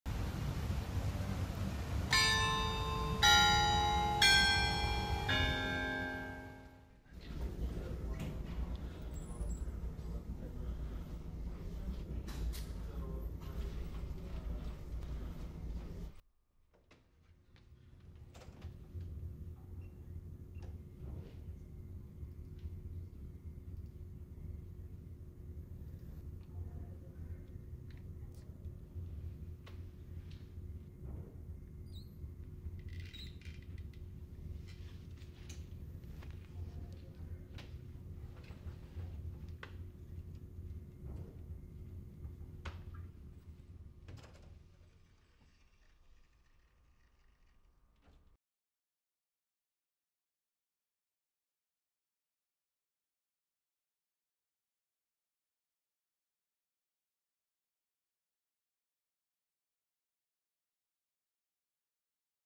CLASS RUSH
A Student realizes he is late to class and hurries to the class room.